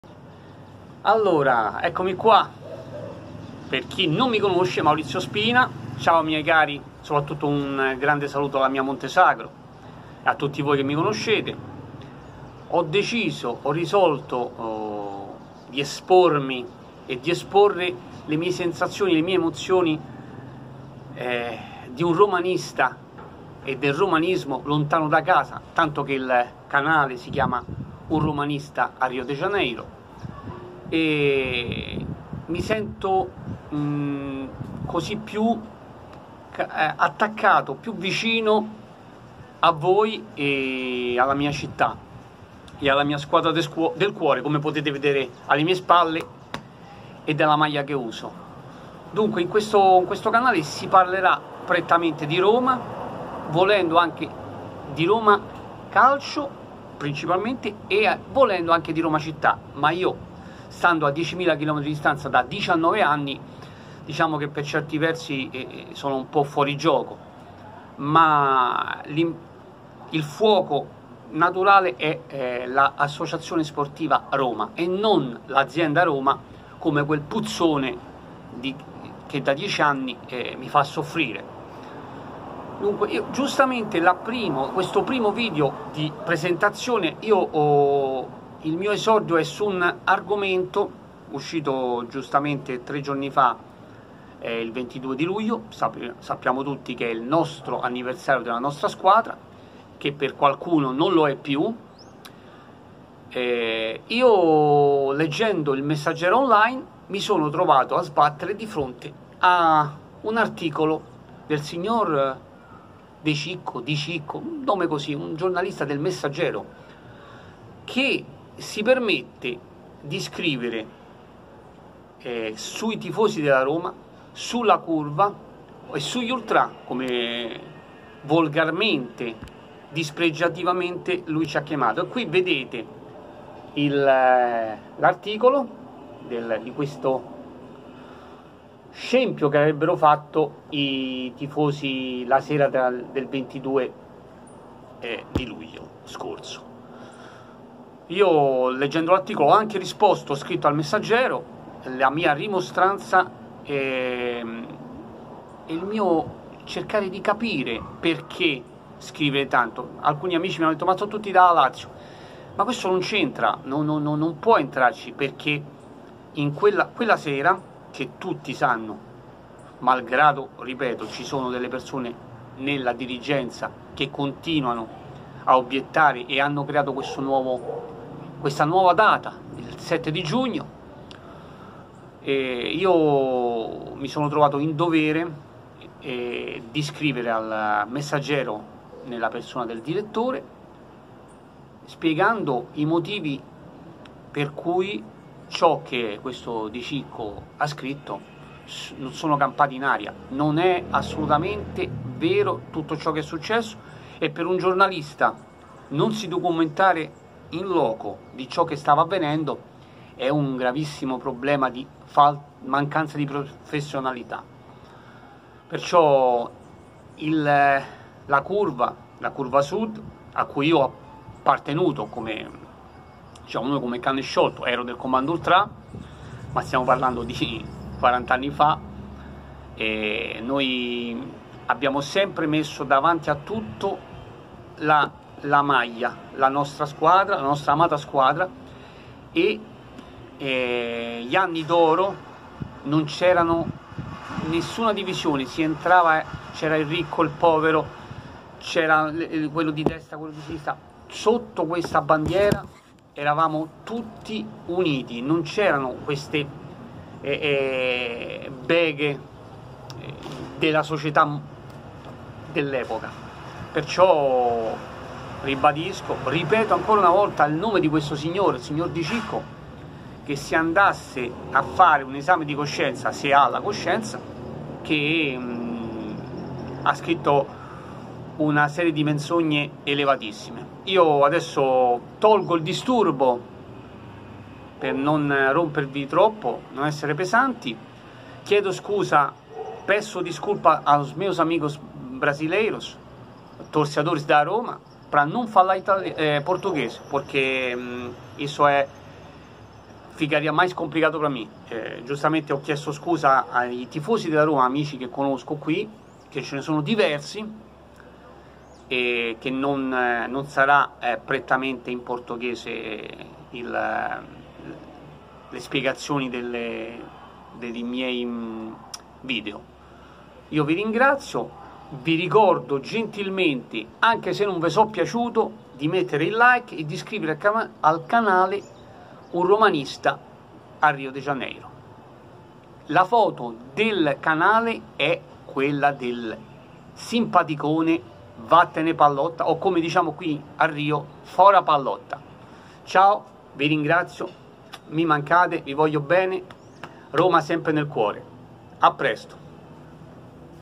Risposta all articolo del sg. De Cicco sul messaggero del 23/7/20
Primo vídeo canale Che nello specifico lá mia risposta a questa specie de giornalista dia cicco Che há letteralmente scritto menzogne sui ragazzi Che hanno organizzare una bellissima festa per lá nostra amata Roma.
Vi parlero' di come e' lá vita di um Romanista a Rio.
Ho pensato di fare cio' per me stesso e per tutti gli amici sparsi spero di Aver trovato una fórmula per sentirvi piu' vicino.